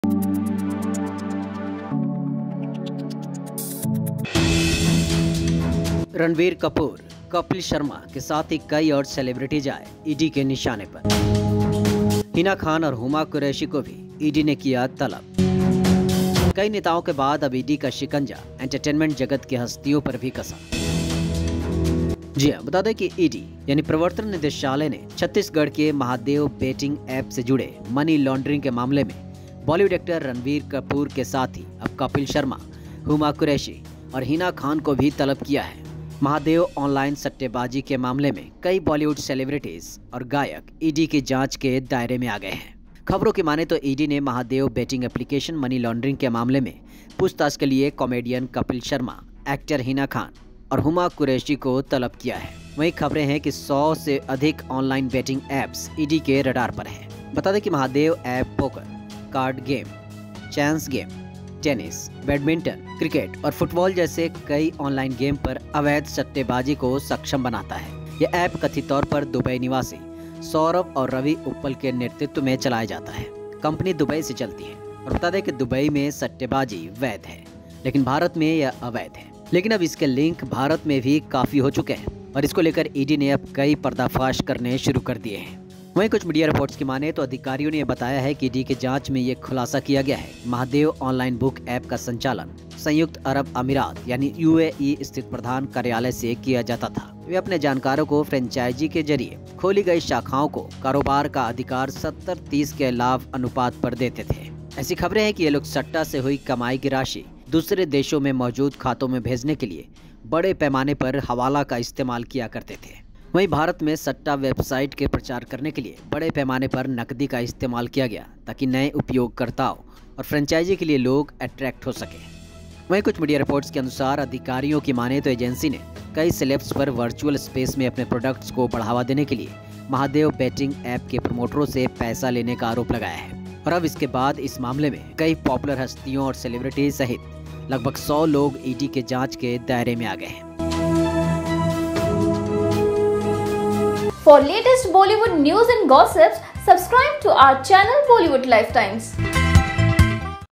Kapil Sharma के साथ Ranbir Kapoor, Hina Khan और Huma Qureshi क्यों आए ED के निशाने पर ?
Kapil Sharma के साथ Ranbir Kapoor, Hina Khan और Huma Qureshi क्यों आए ED के निशाने पर ? ED Summons Kapil Sharma, Huma Qureshi & Hina Khan In Mahadev Betting App Case.